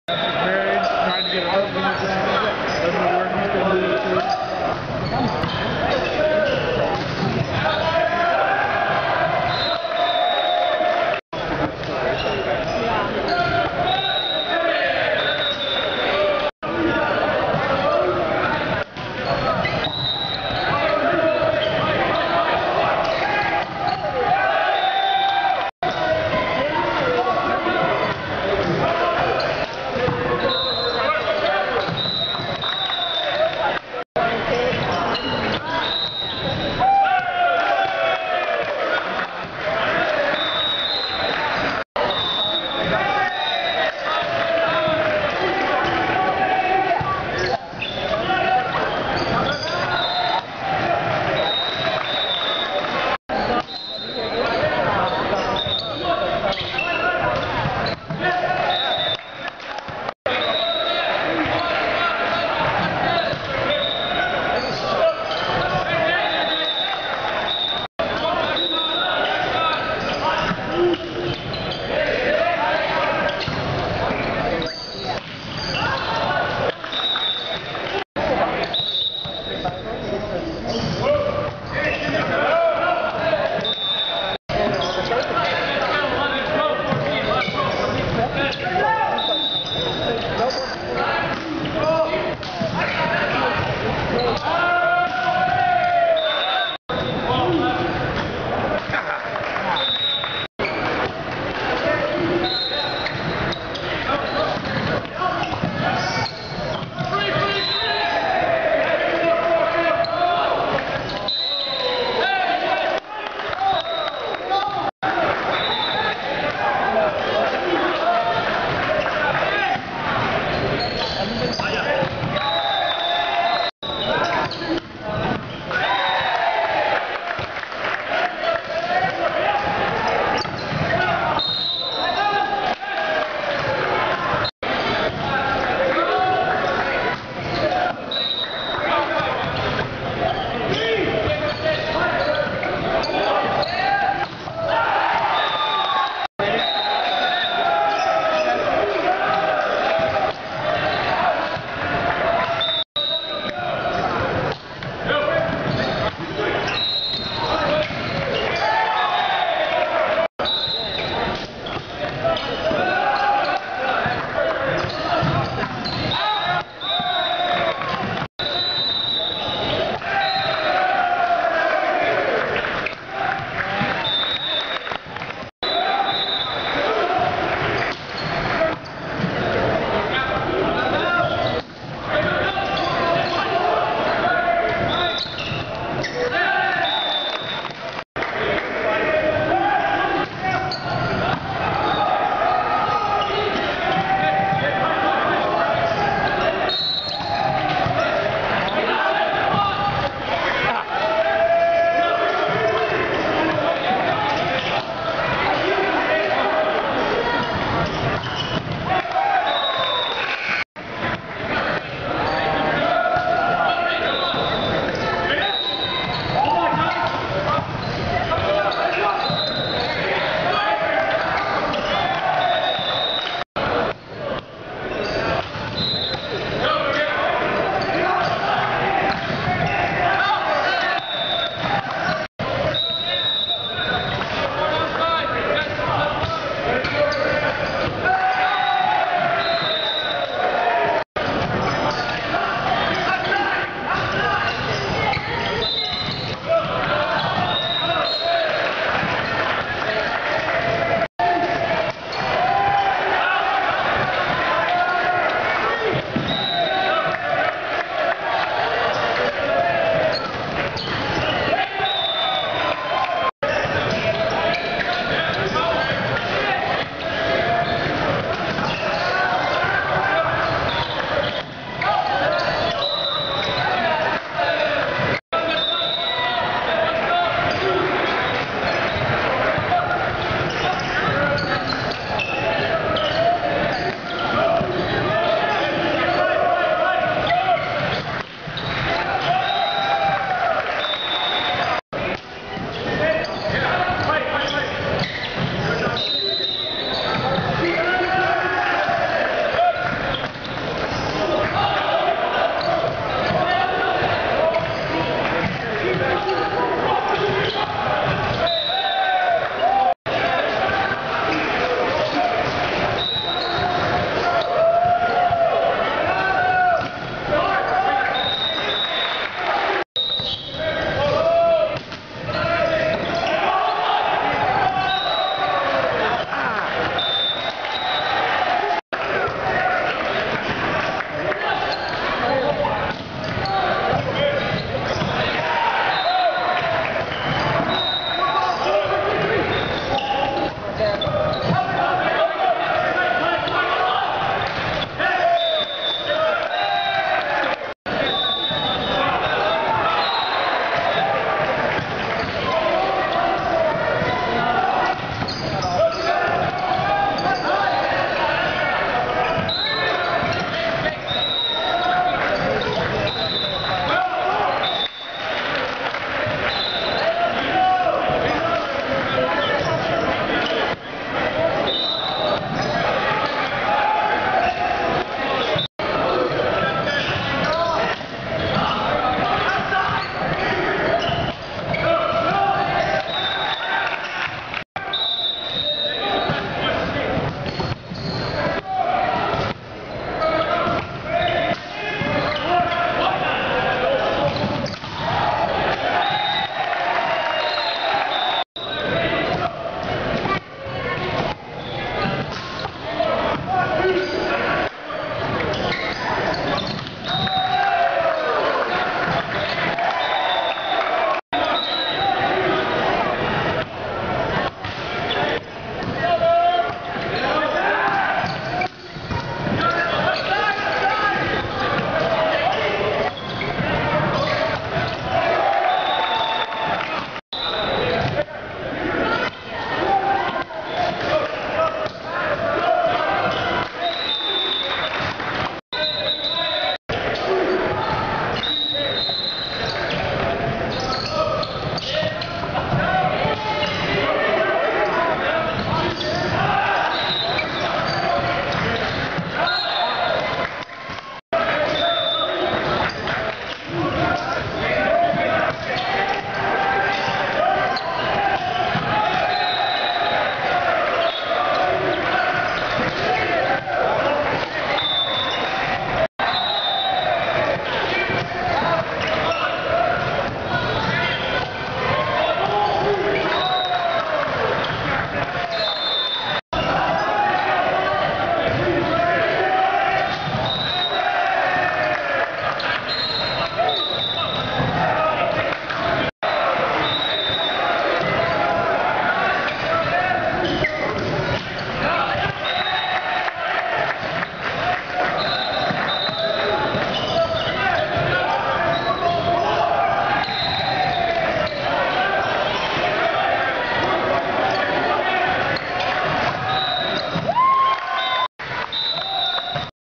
Hilliard Bradley Boys Volleyball 201203243
Pre-season, St Charles Tournament
Saturday tournament play
Match 2  Trinity game 1
Bradley is on the far side in white